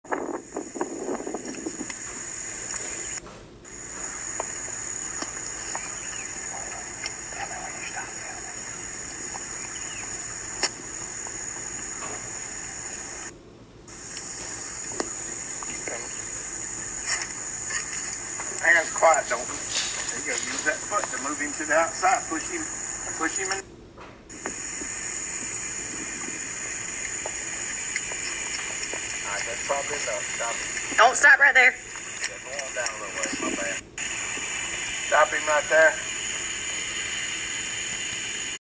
9-7-17 Gage & Red Rocket
Little training pony Gage has in to ride for his 4yo owner!! This is day # 3 with him.